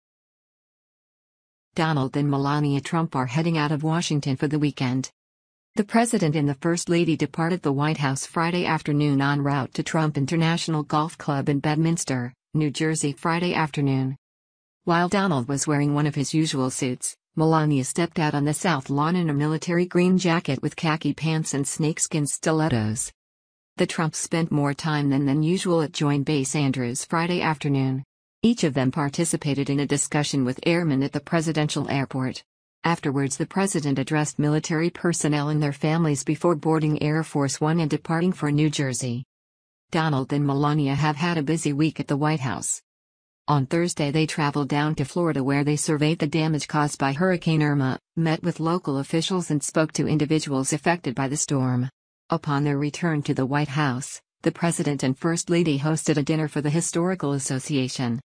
Melania Hollers For Popped Collars
Donald and Melania Trump are heading out of Washington for the weekend.

The president and the first lady departed the White House Friday afternoon en route to Trump International Golf Club in Bedminster, New Jersey Friday afternoon.

While Donald was wearing one of his usual suits, Melania stepped out on the South Lawn in a military green jacket with khaki pants and snakeskin stilettos.

The Trumps spent more time than than usual at Join Base Andrews Friday afternoon. Each of them participated in a discussion with airmen at the presidential airport. Afterwards the president addressed military personnel and their families before boarding Air Force One and departing for New Jersey.

Donald and Melania have had a busy week at the White House.

On Thursday they traveled down to Florida where they surveyed the damage caused by Hurricane Irma, met with local officials and spoke to individuals affected by the storm. Upon their return to the White House, the president and first lady hosted a dinner for the Historical Association.